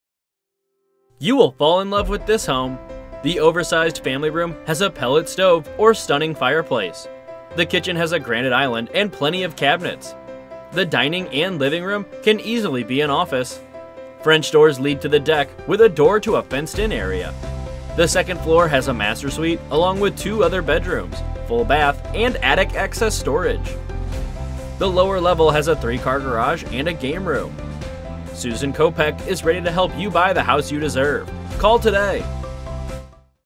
9 Cortland Place Oxford, CT | ColdwellBankerHomes.com
9 Cortland Place, Oxford, CT

This is a home that you will fall in love with the moment you step inside. Our first floor offers a great entertaining open concept with plenty of space for a large gathering. The oversized family room offers a Harmon pellet stove inside or stunning fireplace. Kitchen with granite, island, stainless